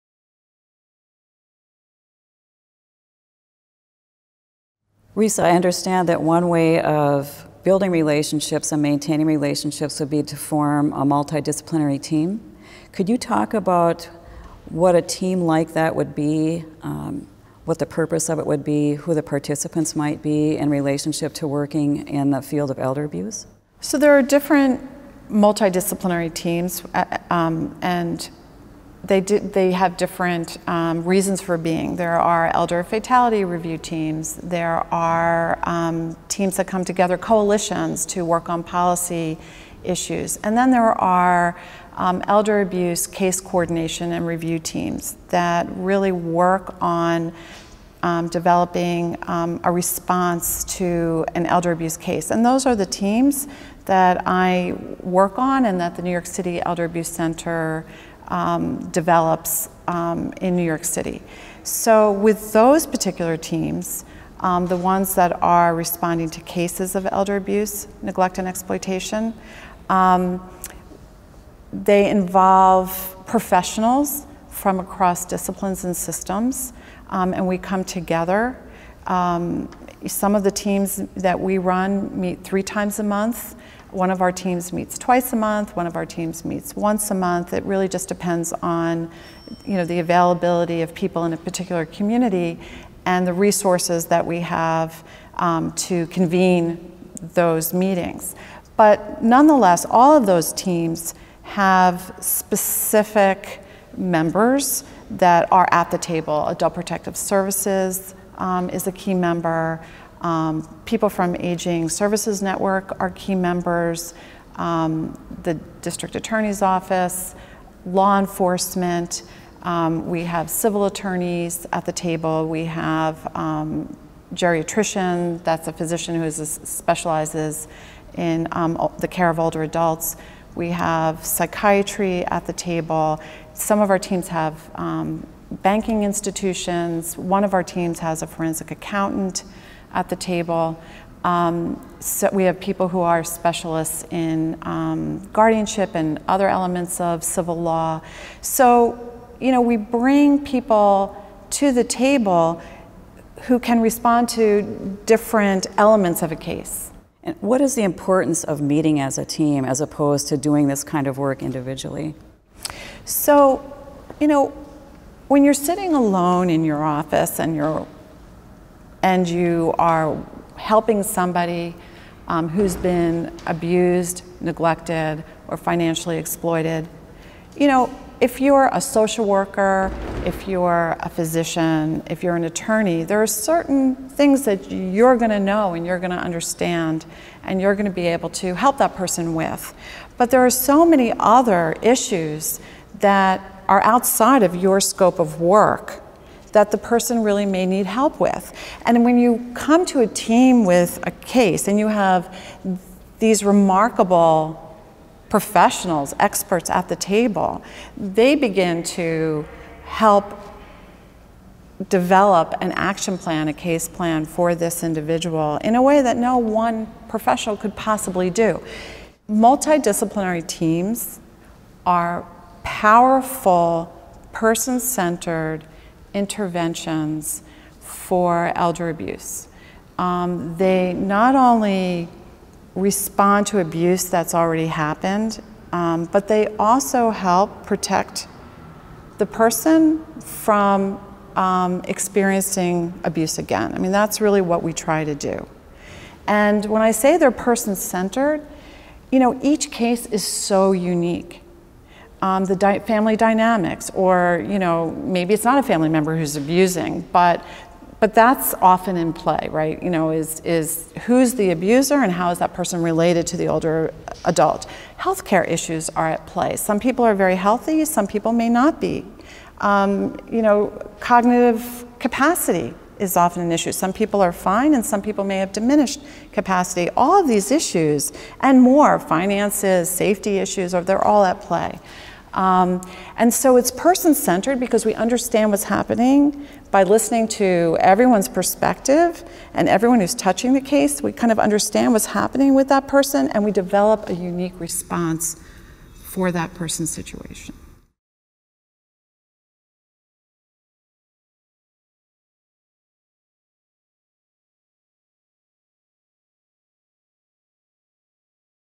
Multidisciplinary Case Coordination and Review Teams (Captioned)
Video presenter: Risa Breckman.

Produced by the National Clearinghouse on Abuse in Later Life (NCALL) and Terra Nova Films. Funded by the U.S. Department of Justice, Office on Violence Against Women and Elder Justice Initiative, 2016.